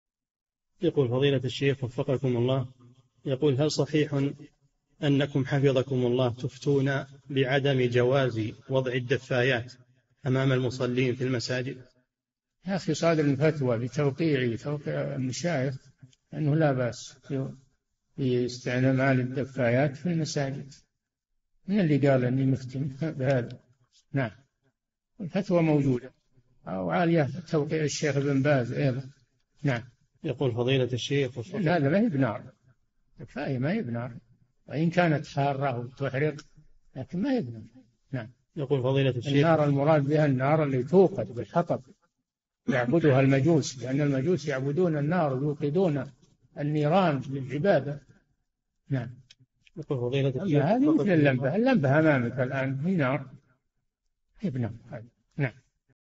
[3623- 4717] هل صحيح أنكم تفتون بعدم جواز وضع الدفايات أمام المصلين في المسجد؟ - الشيخ صالح الفوزان
"[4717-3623] هل صحيح أنكم حفظكم الله تفتون بعدم جواز ووضع الدفايات أمام المصلين في المساجد؟
الجَوَابُ: يا أخي صدر فتوى بتوقيعي وتوقيع المشايخ أنّه لا بأس باستعمال الدفايات في المساجد، من الذي قال أني مُفت بهذا؟ الفتوى موجودة وعليها توقيع الشيخ ابن باز أيضًا؛ لأن هذه ما هي بنار، الدفاية ما هي بنار، وإن كانت حارة وتحرق لكن ما هي بنار، النار المراد بها النار التي توقد بالحطب يعبدها المجوس؛ لأن المجوس يعبدون النار ويوقدون النيران للعبادة، أما هذه مثل اللمبة، اللمبة أمامك الآن هي نار؟ ما هي بنار هذه!

للاشتراك في قناة الشيخ صالح الفوزان
فتاوى المجالس في تفسير المفصل

للاستفسار والإقتراحات والدعم
رئيس الفريق
لمشروع كبار العلماء